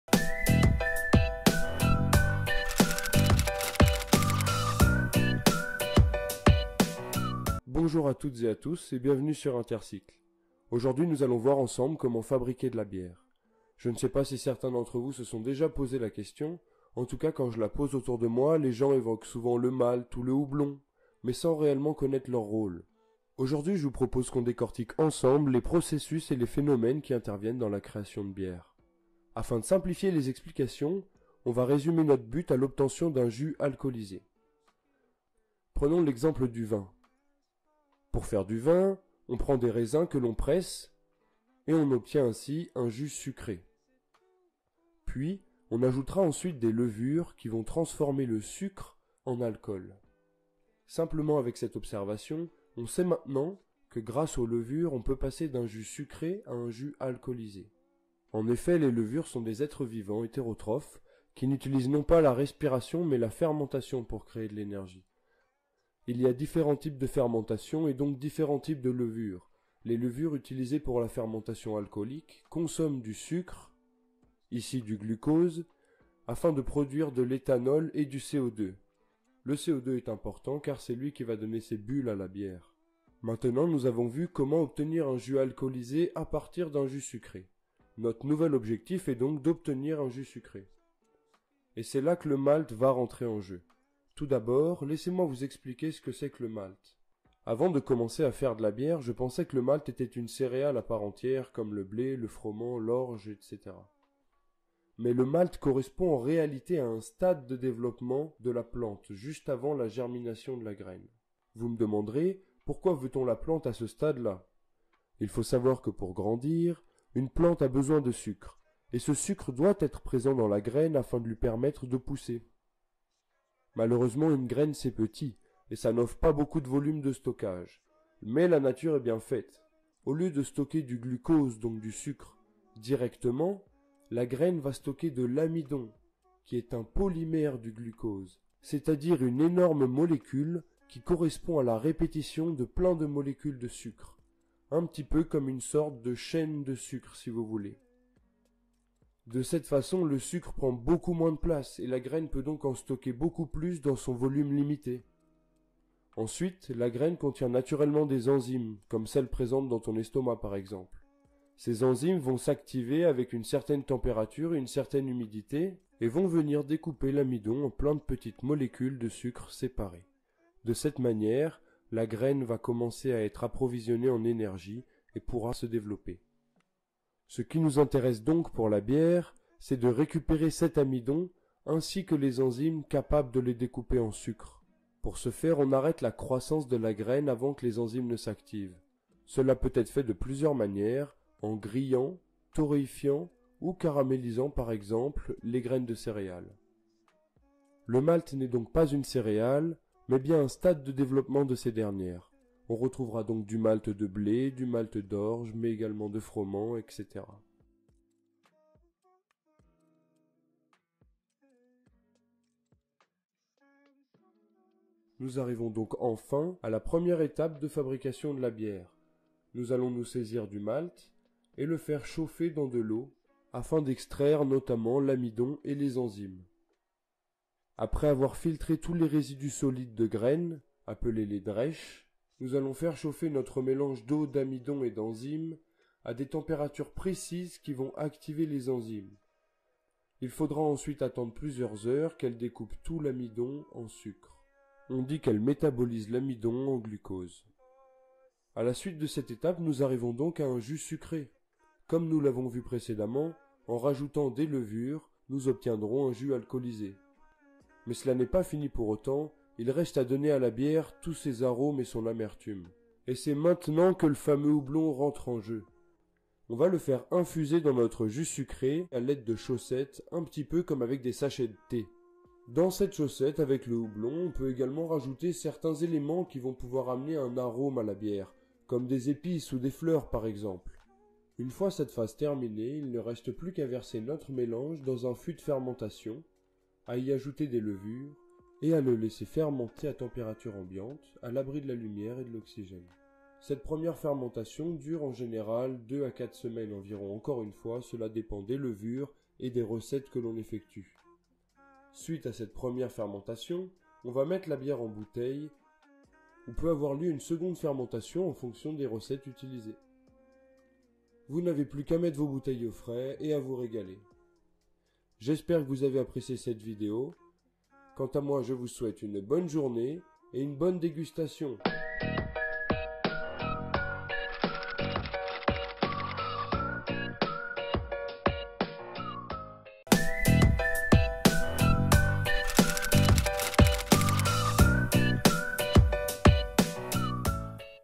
La Bière - Université - InterCycles
Edgar, en école d'ingénieur chimiste, nous explique le processus de fermentation qui mène à la création de la bière, en passant par la composition chimique et les réactions conséquentes!